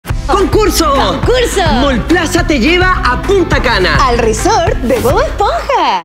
BKNciones en Mallplaza
Ven a vivir tus BKNciones en Mallplaza. Disfruta las Aventuras con Bob Esponja y participa por un viaje a Punta Cana al Resort de Bob Esponja para 4 personas ✈️🌴